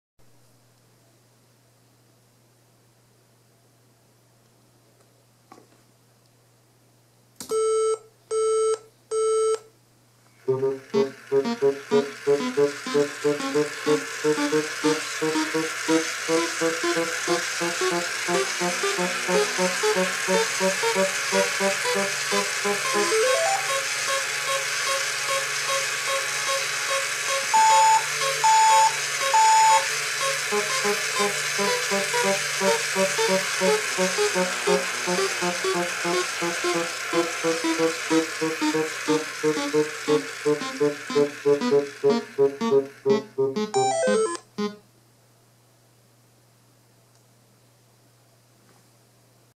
scratch2thymioII fairground
Thymio robot controlled from Scratch.
(uses Scratch2ThymioII)